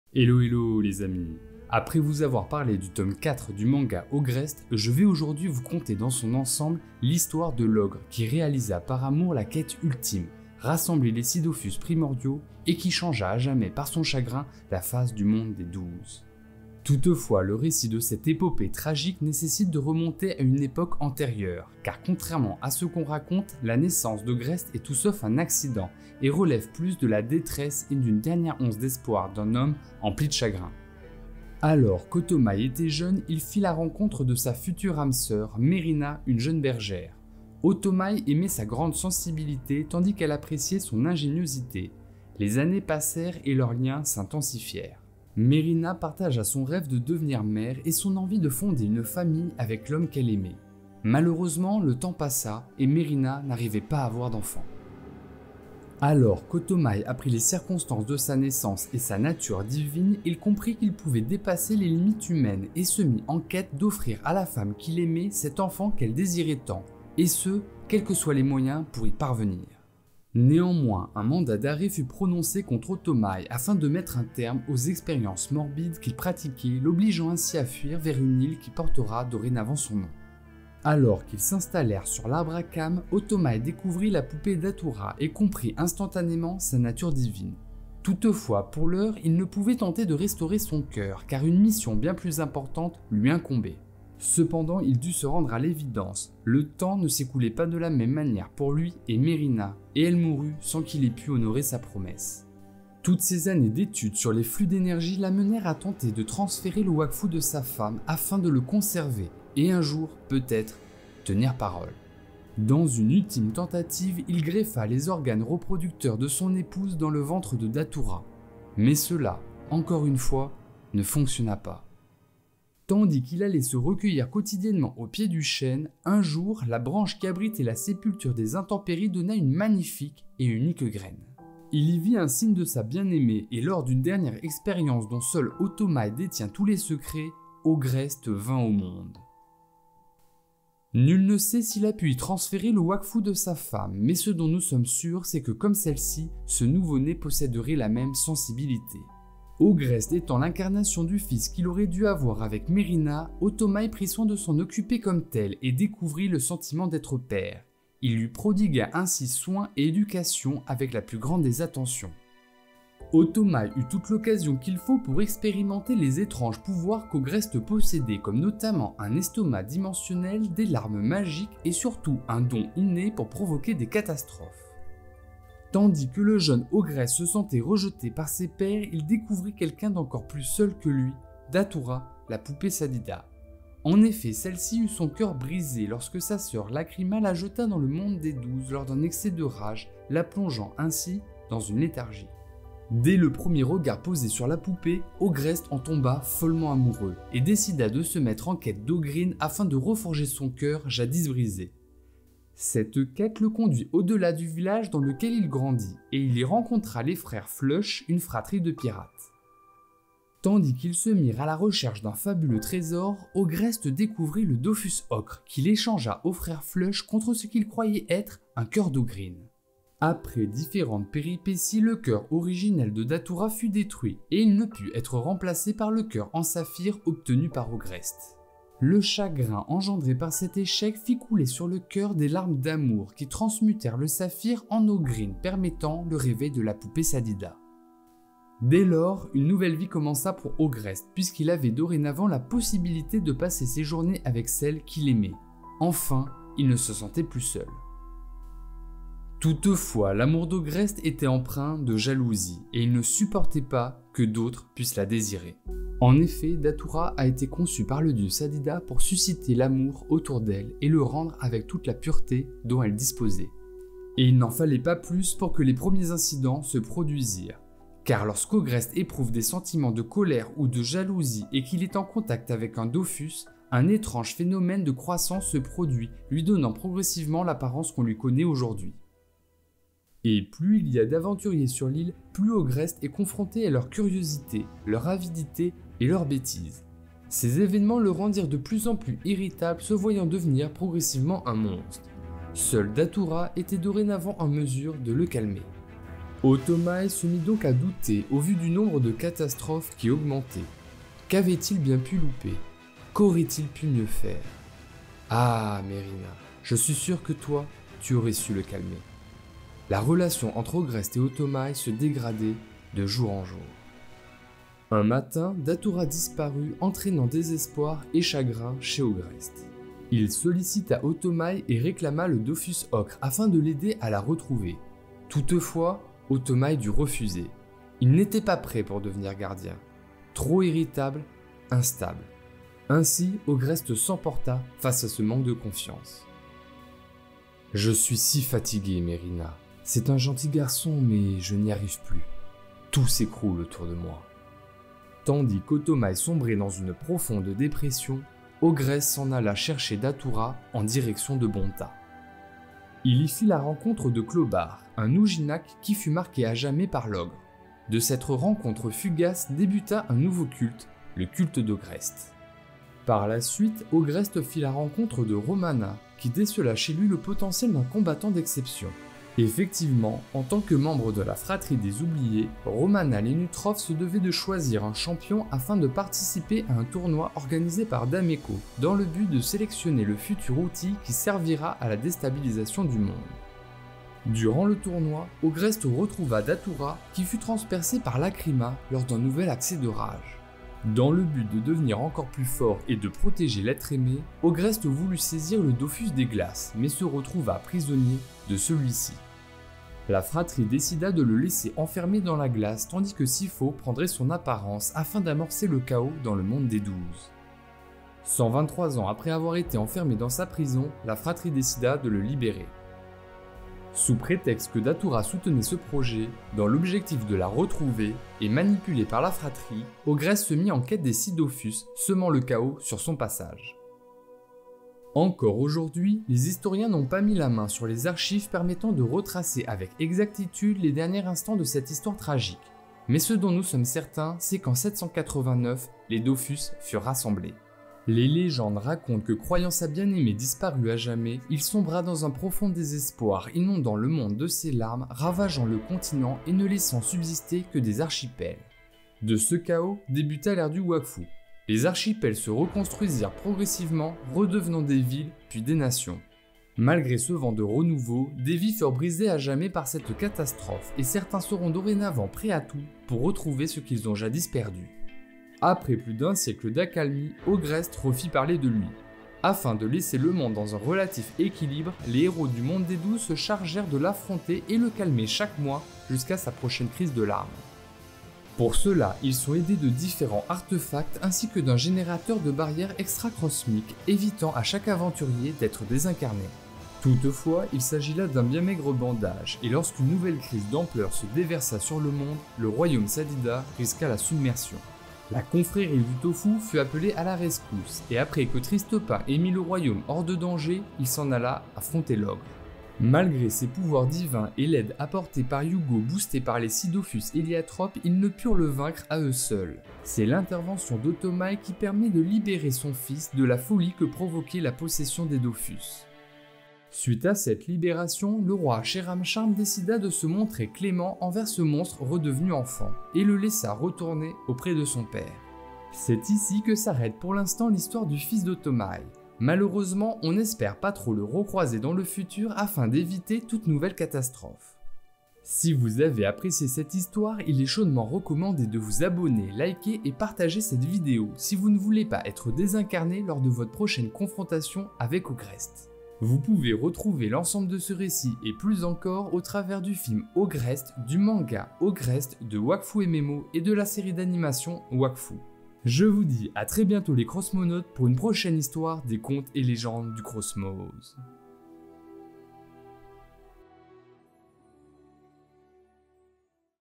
La véritable Histoire d'Ogrest : De l'amour naquit le chaos - Wakfu série
Hello hello les Krosmonautes, aujourd'hui nous allons explorer l'histoire de l'antagoniste de l'ère du Wakfu, Ogrest !!

N'hésitez pas à aller découvrir le manga Ogrest écrit par Mig, ainsi que la série et le jeu pour retrouver tous les éléments comptés :)

Avertissement: Le contenu que je propose n'a pas la prétention d'être le lore officiel du Krosmoz établi par Ankama. Il représente l'état des lieux de mes connaissances et de mes interprétations à un instant T que je partage sous la forme d'une histoire contée.

Je remercie Ankama pour l'Univers qu'ils ont su construire au fur et à mesure et pour le travail qu'ils fournissent afin de l'enrichir.